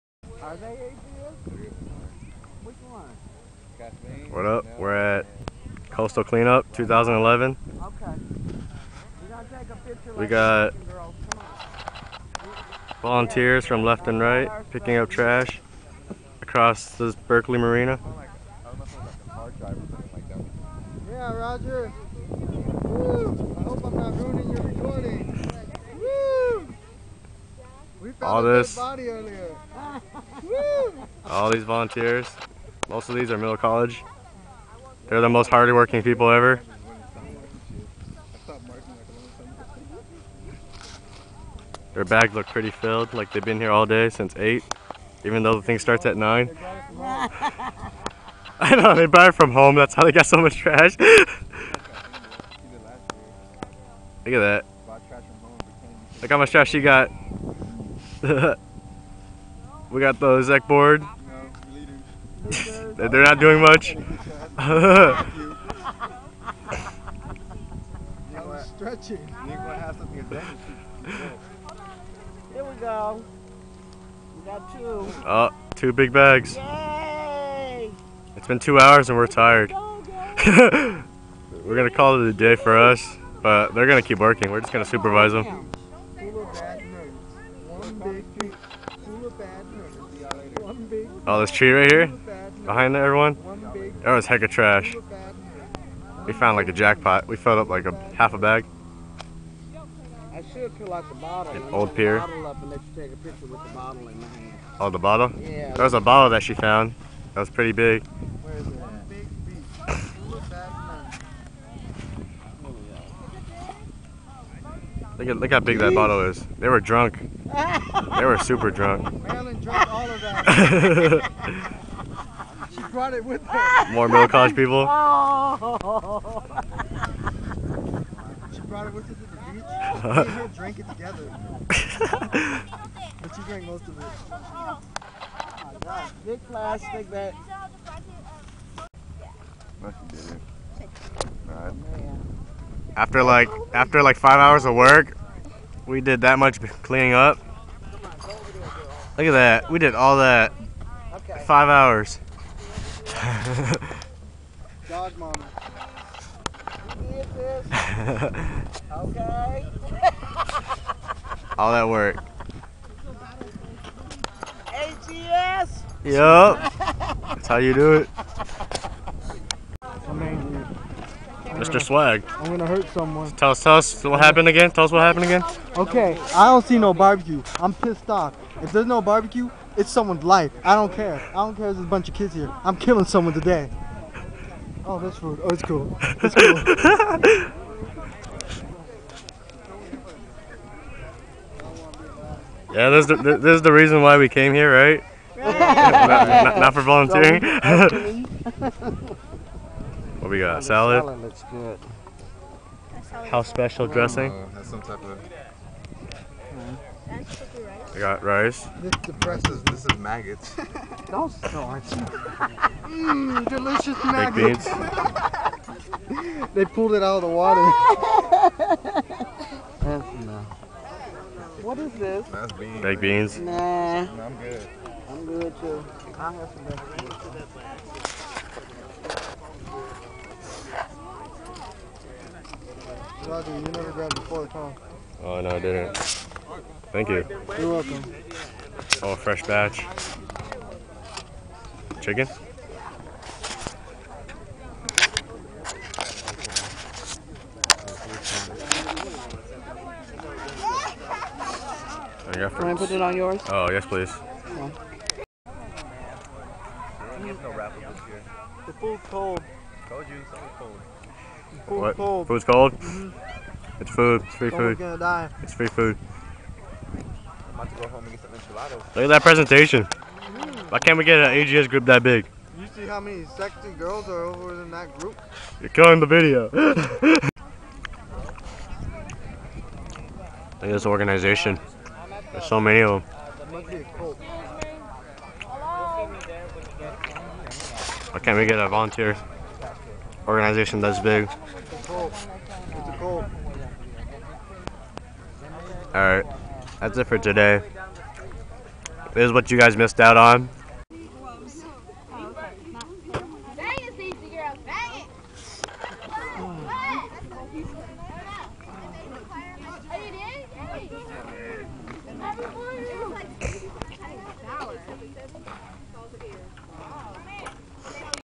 AGS Volunteer Event 2011 Coastal Clean Up Summary Recap
We spent this weekend at the Coastal Clean Up 2011 in the Berkeley Marina with The Watershed Project. California Coastal Cleanup Day, an annual beach and inland waterway cleanup, is the state's largest volunteer event. In 2010, over 82,500 volunteers removed more than 1.2 million pounds of trash and recyclables from our beaches, lakes, and waterways.

The Watershed Project's goal is havingpeople committed to a healthy San Francisco Bay watershed. Since 1997, they have pursued that dream by:

1)   Restore and preserve our unique, local ecosystems
2)   Give students, teachers and volunteers the tools to preserve healthy watersheds
3)   Bring a watershed perspective to the urban environment
4)   Support grassroots watershed organizations
5)   Nurture young people who aspire to environmental careers working for positive change in our Bay Area communities

Alpha Gamma Sigma Honor Society at Contra Costa College (CCC).